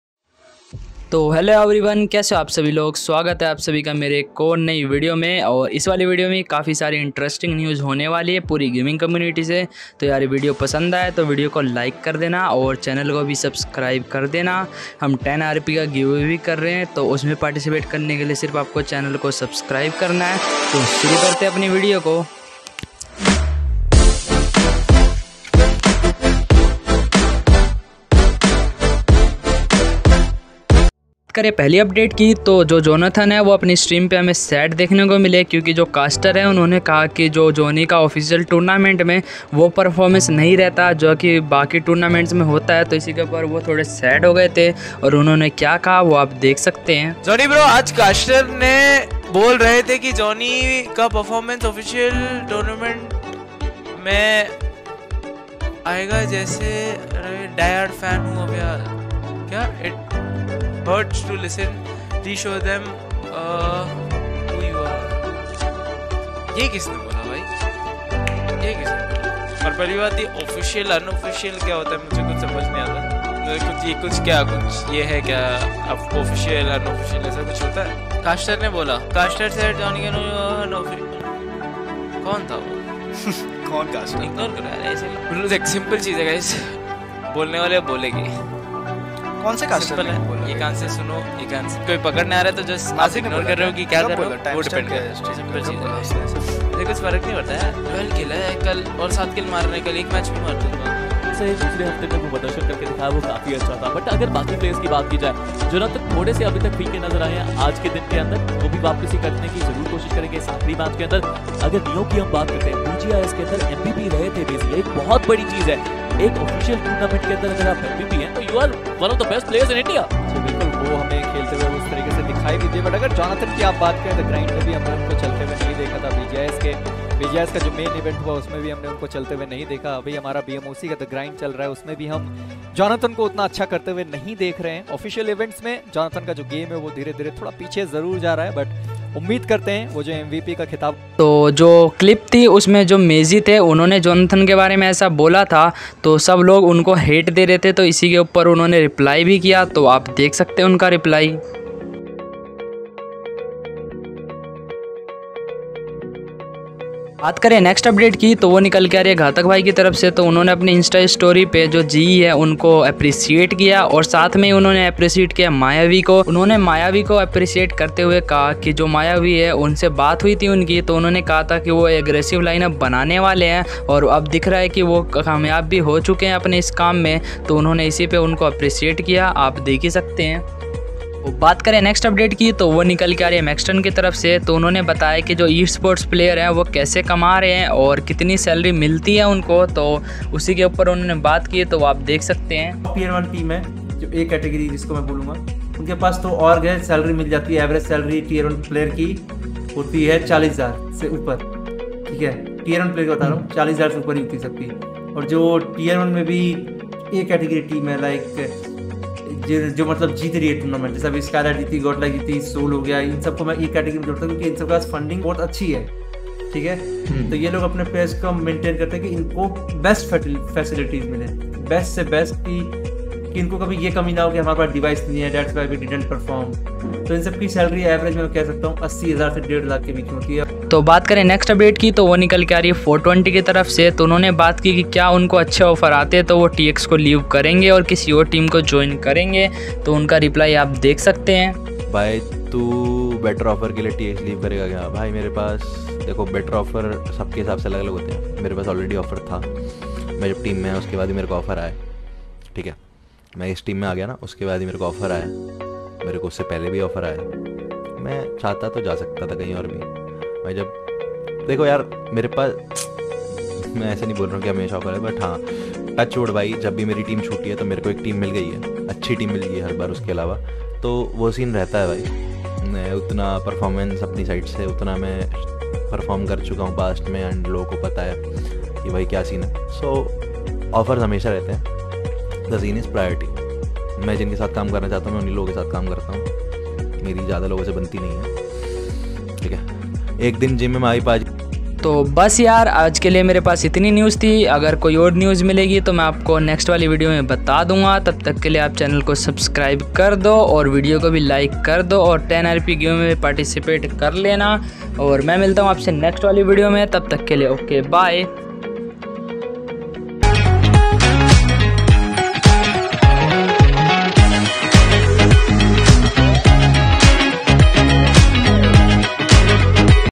BGMI NEWS:-T1 PLAYERS SALARY || TX NEW JERSEY || MAZY REPLY TO JONATHAN || GHATAK ON GE
time stamps
00:00 intro
00:31 jonathan and mazy matter
02:22 mazy reply
02:41 Ghatak appreciate ge
03:03 t1 player salary
03:52 420 on leaving tx
04:50 10 RP GIVEAWAY 🔥
05:10 THANKS FOR WATCHING ❤️